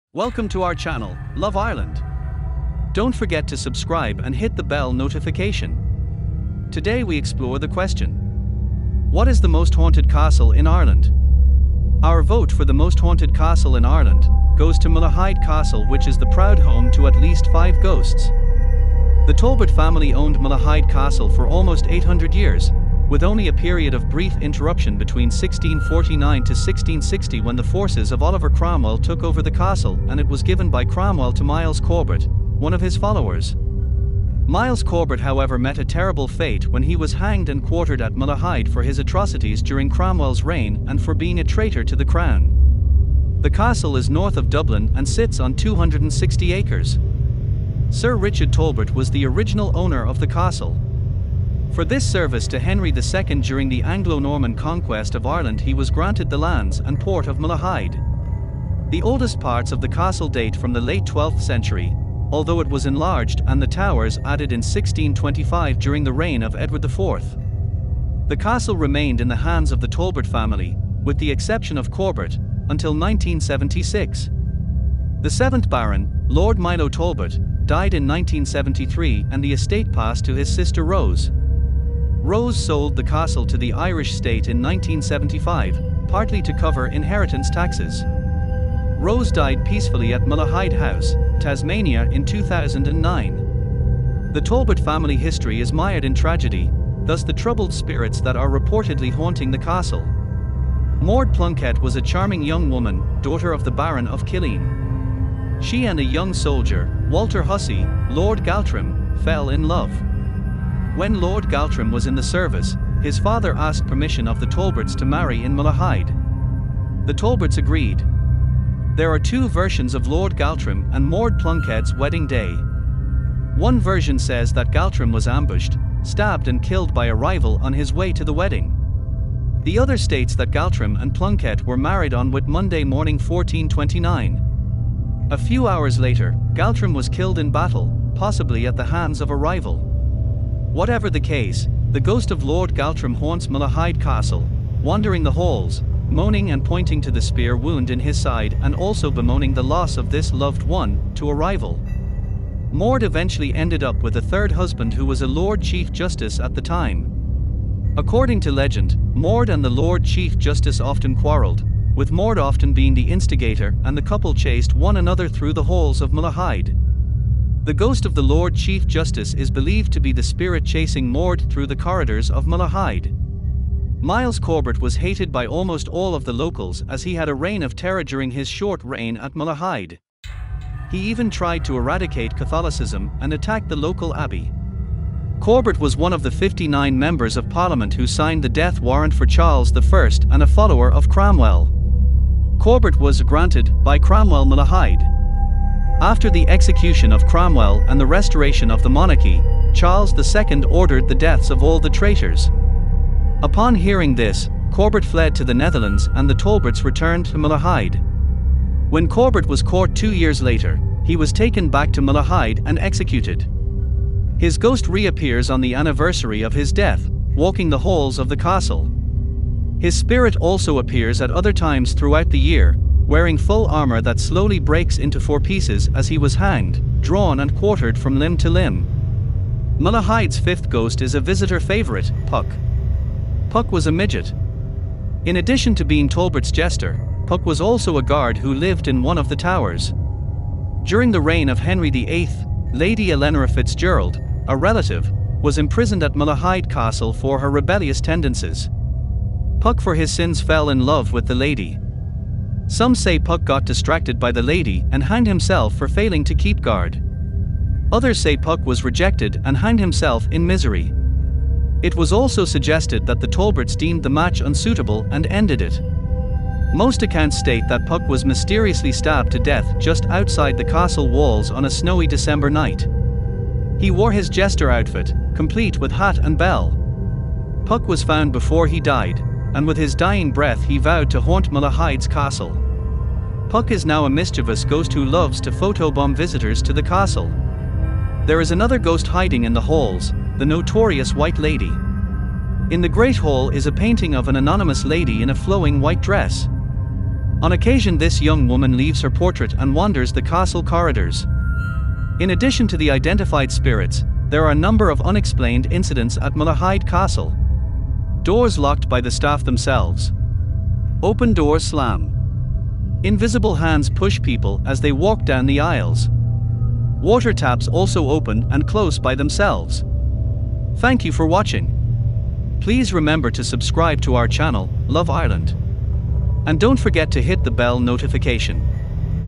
What is the most haunted castle in Ireland?
Our vote for the most haunted castle in Ireland, goes to Malahide Castle which is the proud home to at least five ghosts.

The Talbot family owned Malahide Castle for almost 800 years, with only a period of brief interruption between 1649 to 1660 when the forces of Oliver Cromwell took over the castle and it was given by Cromwell to Miles Corbet, one of his followers.

Miles Corbett however met a terrible fate when he hanged and quartered at Malahide for his atrocities during Cromwell’s reign and for being a traitor to the crown.

The Castle is north of Dublin and sits on 260 acres. Sir Richard Talbot was the original owner of the castle. For this service to Henry II during the Anglo-Norman conquest of Ireland he was granted the lands and port of Malahide. The oldest parts of the castle date from the late 12th century, although it was enlarged and the towers added in 1625 during the reign of Edward IV.

The castle remained in the hands of the Talbot family, with the exception of Corbett, until 1976.